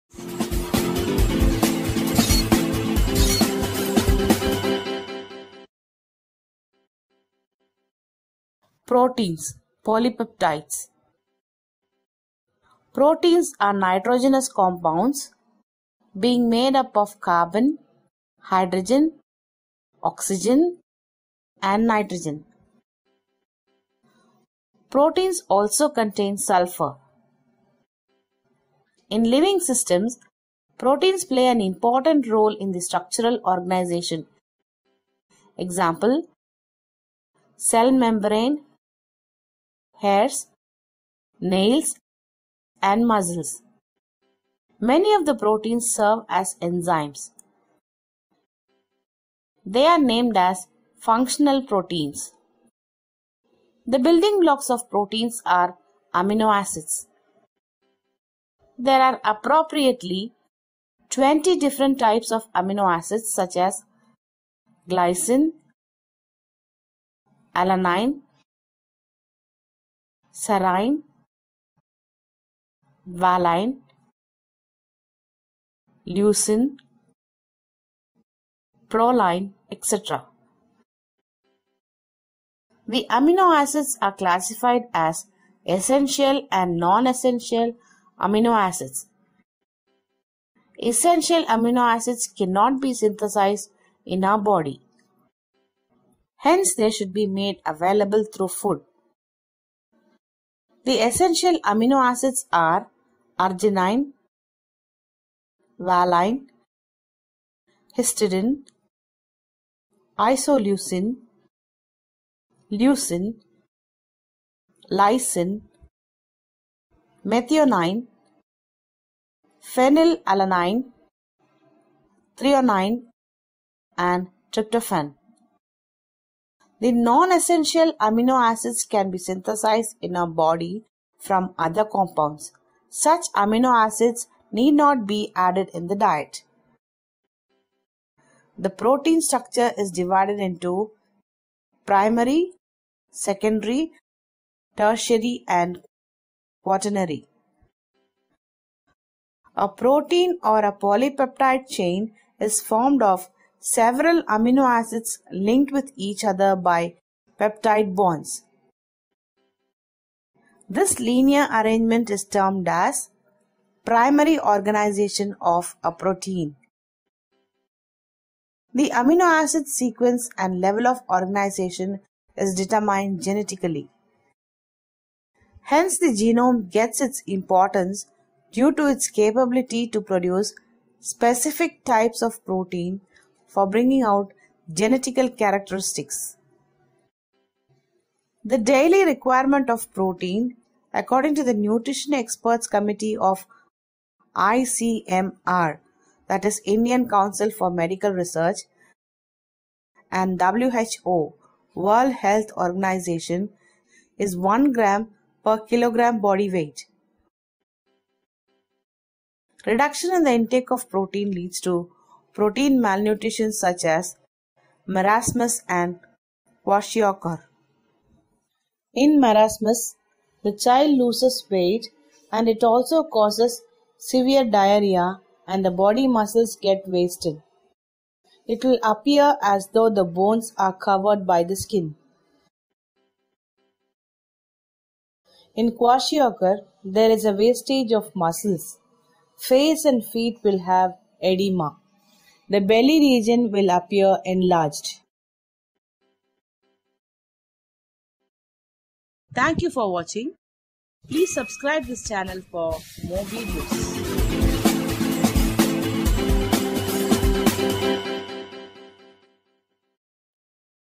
What Are Proteins (Polypeptides)? | Deficiency, requirements and Sources
Proteins are nitrogenous compounds being made up of carbon, hydrogen, oxygen and nitrogen. Proteins also contain sulphur. In living systems proteins play an important role in the structural organisation eg., cell membrane, hairs, nails and muscles. Many of the proteins serve as enzymes. They are named as functional proteins.

The building blocks of proteins are aminoacids. There are approximately twenty different types of amino acids, such as glycine, alanine, serine, valine, leucin, proline etc.

The amino acids are classified as essential and non essential amino acids. Essential amino acids can not be synthesised in our body. Hence they should be made available through food.

The essential amino acids are arginine, valine, histidine, isoleucine, leucine, lycine, methionine, phenylalanine, threonine and tryptophan. The nonessential amino acids can be synthesized in our body from other compounds. Such amino acids need not be added in the diet.

The protein, structure is divided in to primary secondary, tertiary and quaternary

A protein (or) a polypetide chain is formed of several amino acids linked with each other by peptide bonds. This linear arrangement is termed as primary organisation of a protein.

The amino-acids sequence and level of ogranisation is determined genetically.

Hence the genome gets its importance due to its capability to produce, specific types of protein for bringing out genetical characteristics.

The daily requirement of protein, according to the Nutrition experts committee of ICMR (Indian Council for Medical Research) and WHO (World Health Organisation) is 1 gm per kg body weight. Reduction in the intake of protein leads to protein malnutritions, such as marasmus and kwashiorkar.

In marasmas the child loses weight and it also causes severe diarrhoea and the body muscles get wasted. It will appear as though the bones are covered by the skin. In kwashiorkar there is a wastage of muscles. Face and feet will have oedema. The belly region will appear enlarged.